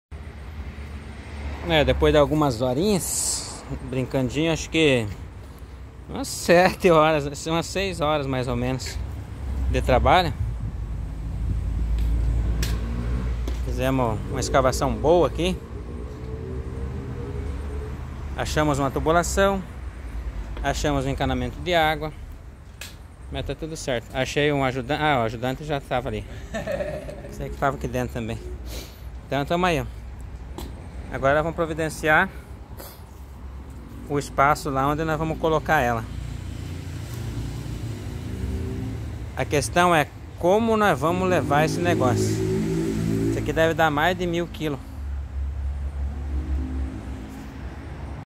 Remoção de jabuticabeira gigante, replantar jabuticabeira. vídeo 2
Remoção de jabuticabeira, replantar jabuticabeira.

olá amigos neste vídeo mostro a vocês a sequência da remoção da jabuticabeira. o torrão que deixei pra querela não morra.

deixem seu comentário
deixem seu joinha